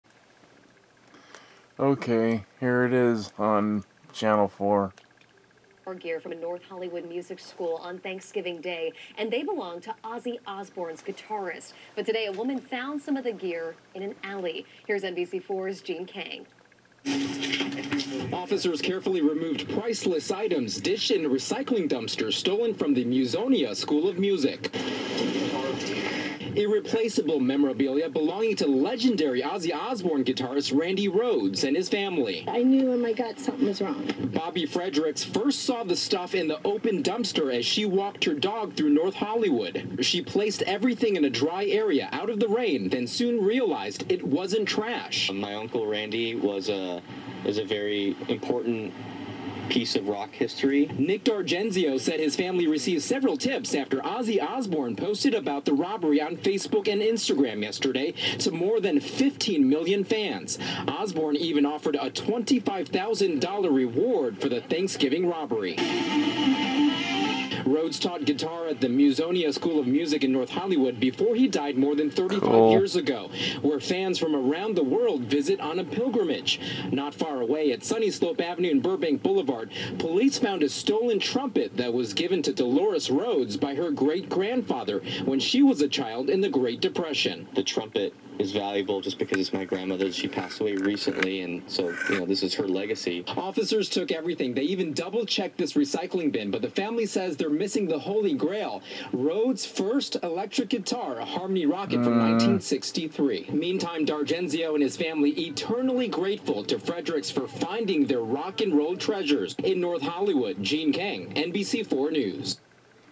RANDY RHOADS & DELORES STOLEN ITEMS FOUND!!!!!!!! EXCEPT THE...
.FIRST GUITAR OF RANDY'S!!!
VERY ,VERY GOOD,BUT KINDA ..WEIRD ,ANYWAY,I HAVE NO COMMENT,TOO MANY PEOPLE WITH TOO MUCH FREE TIME! SO CHECK IT OUT! SO THIS IS A PART 3 OF MY 3 PART RANDY RHOADS BIRTHDAY VIDEO ,WITH A NICE ENDING!